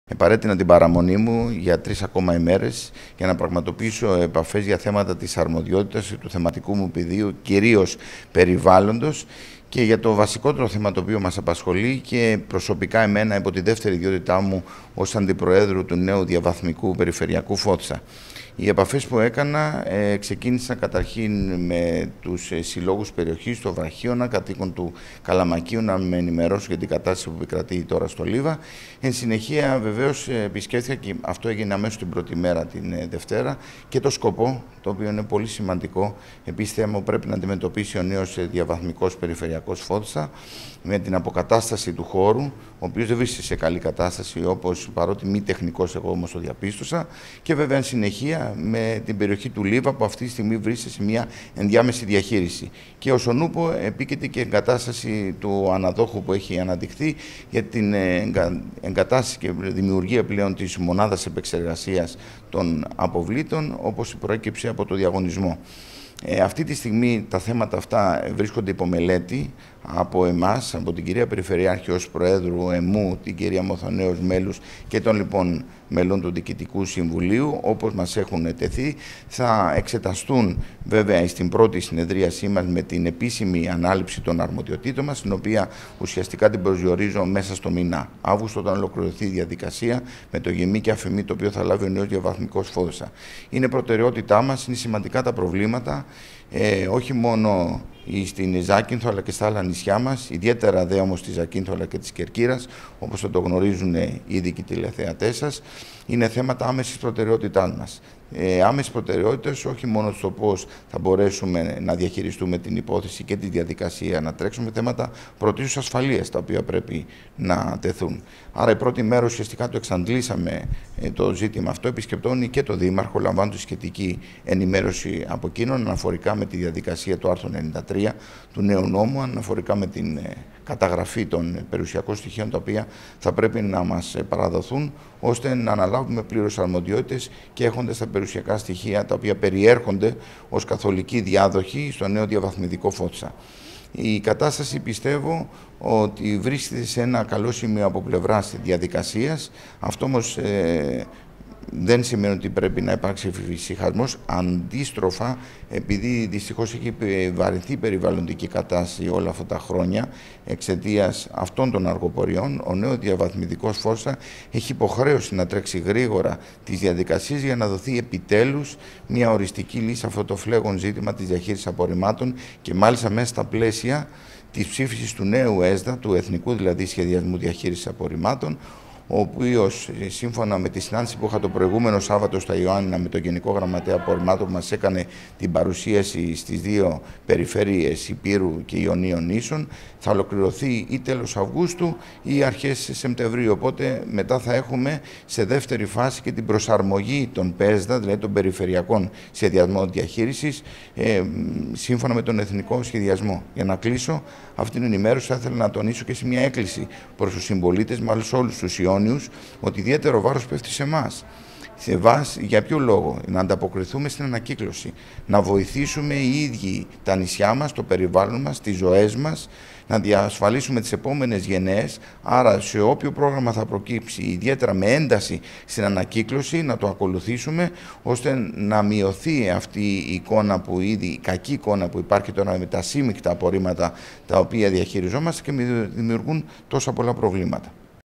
Συναντήσεις και επαφές του Αντιπεριφερειάρχη Περιβάλλοντος στη Ζάκυνθο
Συναντήσεις και επαφές του Αντιπεριφερειάρχη Περιβάλλοντος στη Ζάκυνθο | Στο επίκεντρο η διαχείριση των απορριμμάτων και ο νέος περιφερειακός ΦΟΔΣΑ

Σπύρος Ιωάννου - Αντιπεριφερειάρχης Περιβάλλοντος, Ενέργειας & Χωροταξίας Ι.Ν.